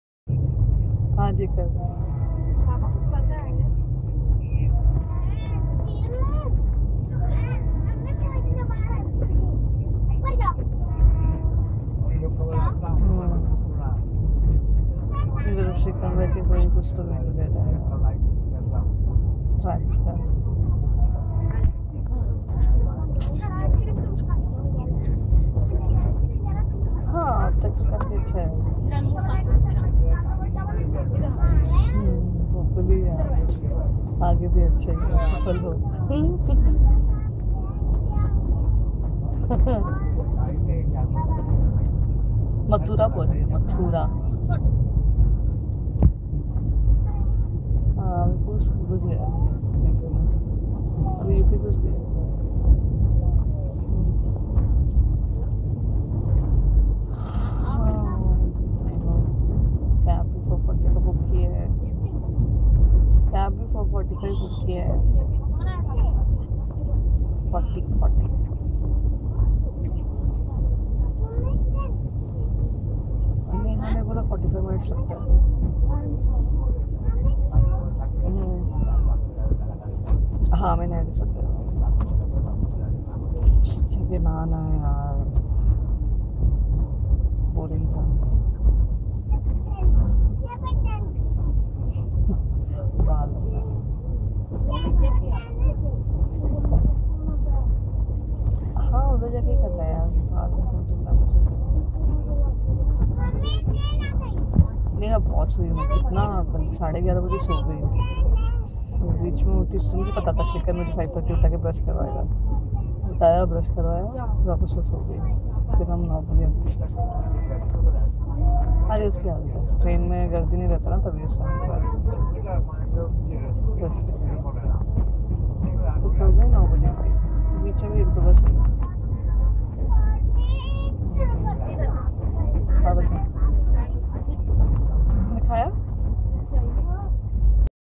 NDLS Duronto passing though Mathura Junction
BRC WAP-5 # 30049 led 22209 BCT - NDLS Duronto skips Mathura Junction (MTJ) as it approaches the last round of its journey, a SBI WDG-4 can be spotted along with an unknown WDS-6 at MTJ outer. VTA WDG-3A # 13485 with a freight load (could not see the rakes) and BGKT WDG-4 # 12584 hauled BOXN rakes and an unknown train led by TATA WAP-7 # 27534 were spotted at MTJ station. Also, LDH WAG-7 # 24681 and TKD WAG-9 # 31185 were spotted resting beside the freight load.  As we left MTJ,  we crossed another set of BOXN rakes led by SBI WDG-4 # 12249.

Video captured on 09/07/2013